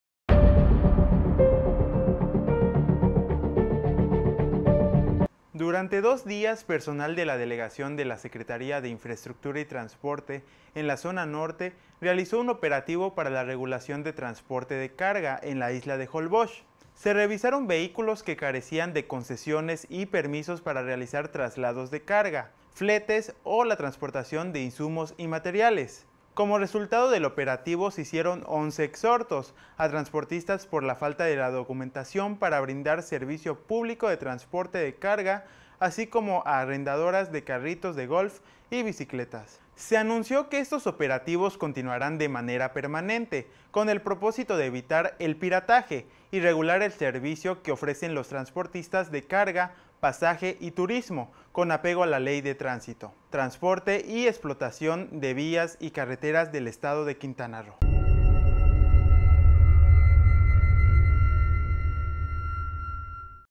REALIZA LA SINTRA OPERATIVO DE INSPECCIÓN EN LA ISLA DE HOLBOX
Entérate de las noticias “EN CORTO”…REALIZA LA SINTRA OPERATIVO DE INSPECCIÓN EN LA ISLA DE HOLBOX…